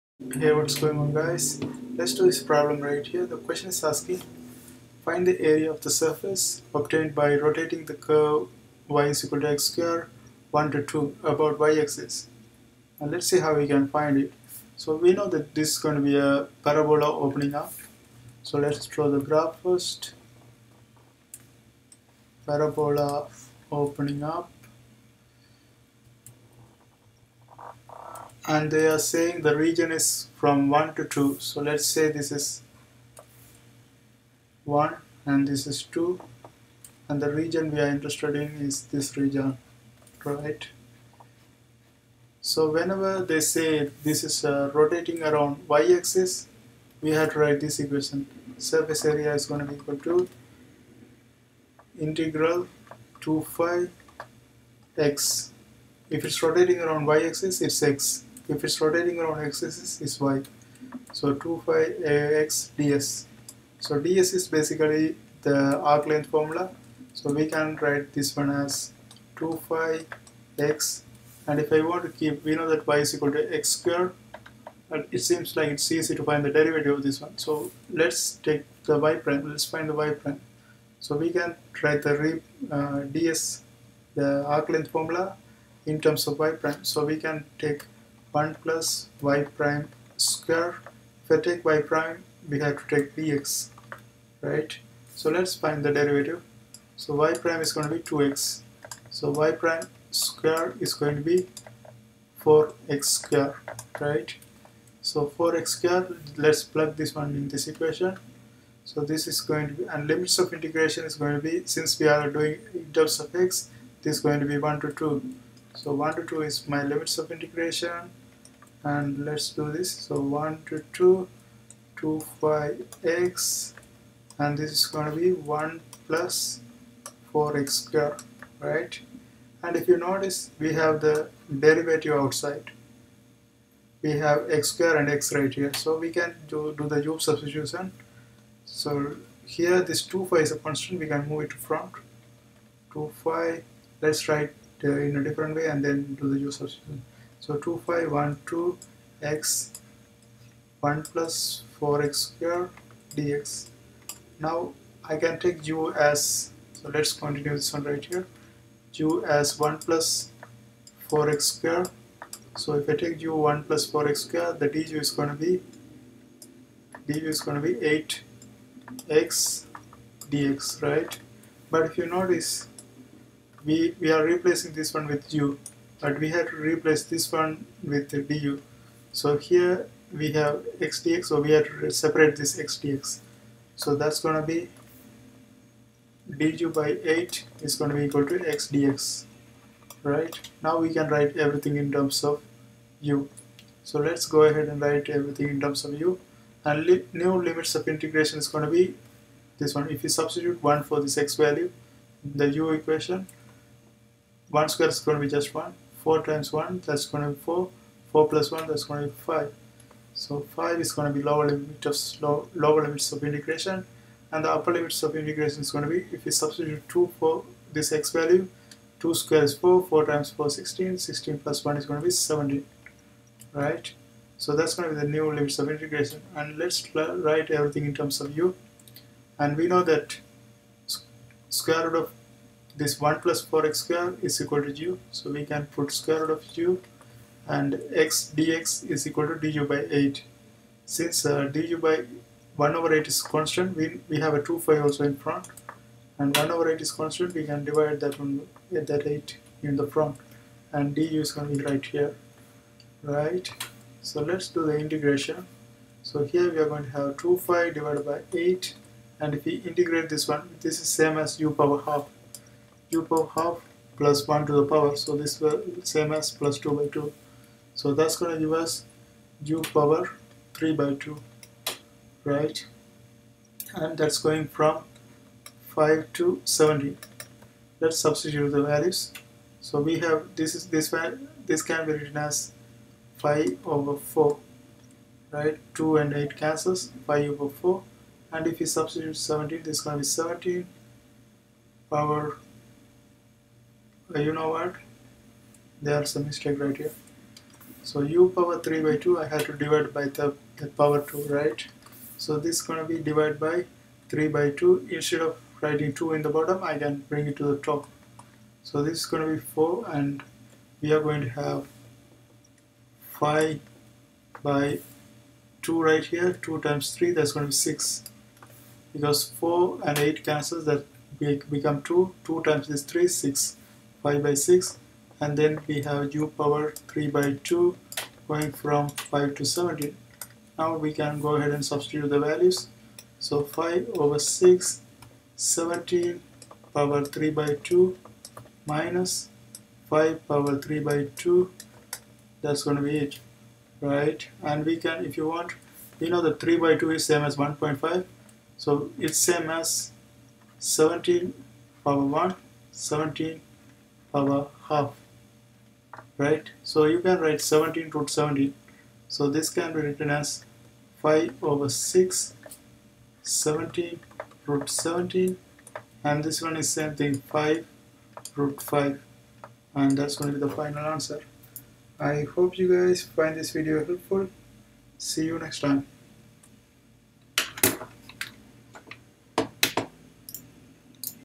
Find the area of the surface obtained by rotating the curve y = x2, 1 ≤ x ≤ 2 about the y axis
Find the area of the surface obtained by rotating the curve y = x2, 1 ≤ x ≤ 2 about they-axis